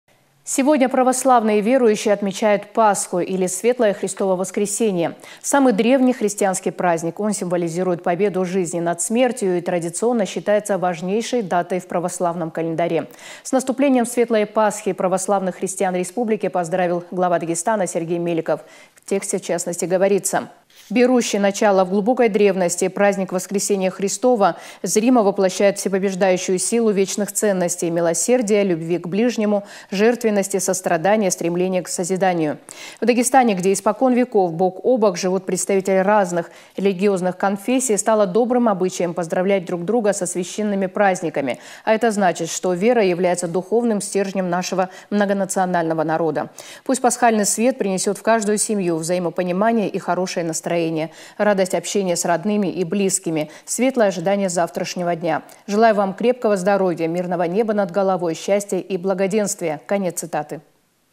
Глава Дагестана поздравил православных христиан республики с Пасхой
Сегодня православные верующие отмечают Пасху, или Светлое Христово Воскресение — самый древний христианский праздник. Он символизирует победу жизни над смертью и традиционно считается важнейшей датой в православном календаре. С  наступлением светлой Пасхи православных христиан республики поздравил Глава Дагестана Сергей Меликов.